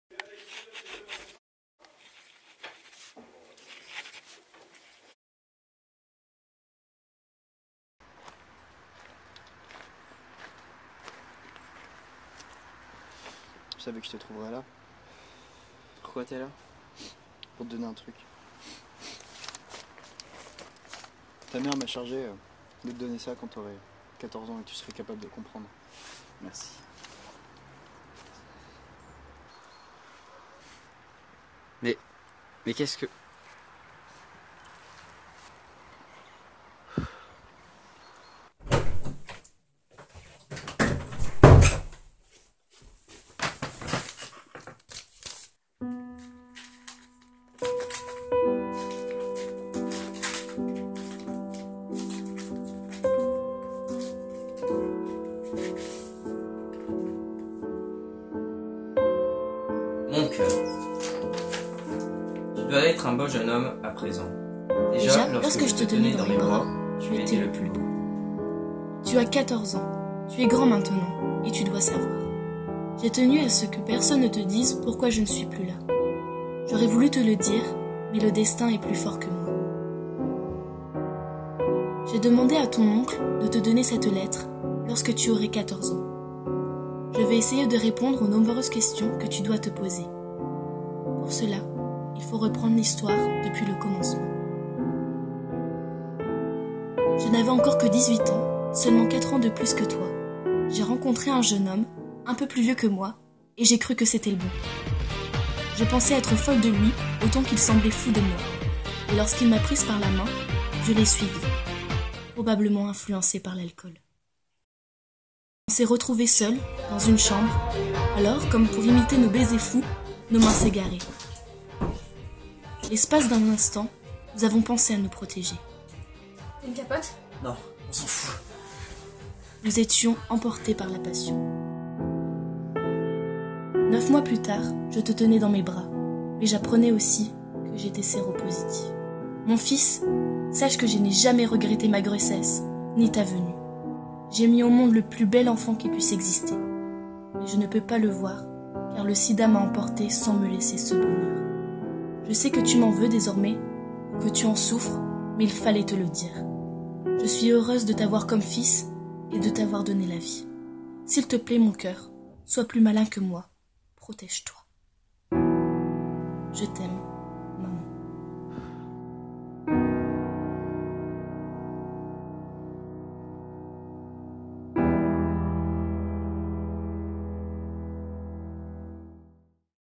Mon cœur - VIH Pocket Film
Vidéo réalisée pour le concours "VIH Pocket Film" organisé par l'association Sidaction en 2016.